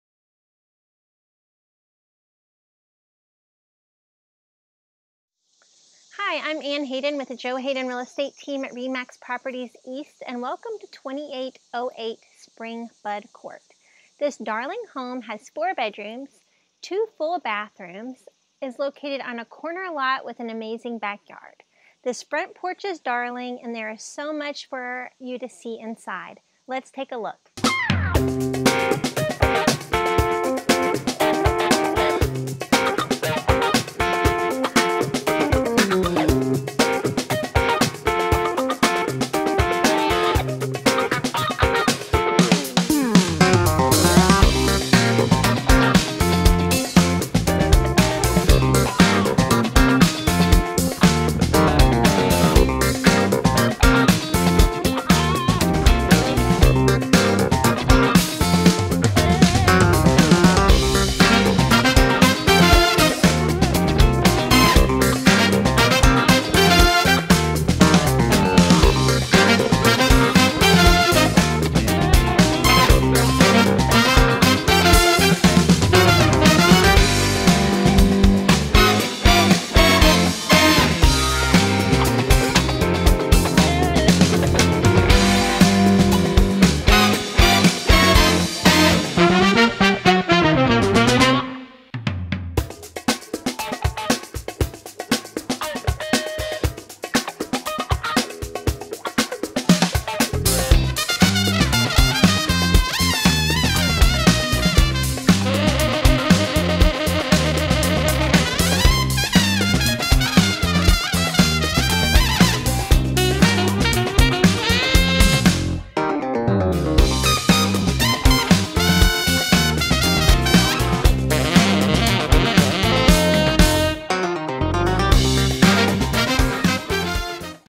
Your New Home! - 2808 Spring Bud Ct Louisville KY
Welcome home to 2808 Spring Bud Court! Located in the desirable Four Seasons neighborhood this home offers 4 bedrooms and 3 full bathrooms. The great room is greets you as you enter the open and features easy access to the kitchen and dining room. The kitchen offers plenty of cabinet and counter-space and gives you access to the deck and spacious backyard. The family room has a beautiful fireplace. One bedroom, full bathroom and laundry are also found on the 1st level with the family room. 3 additional bedrooms are found on the upper level including the primary bedroom. The primary bedroom has vaulted ceilings and an en-suite bathroom. The 3rd full bathroom is centrally located with the bedrooms on the upper level. The spacious 2.5 car attached garage has plenty of room to keep your vehicles out of the weather and offers room for additional storage. 2808 Spring Bud Court gives you all of the conveniences you need by being close to the expressway, shopping and dining. Schedule your personal showing today before it's gone!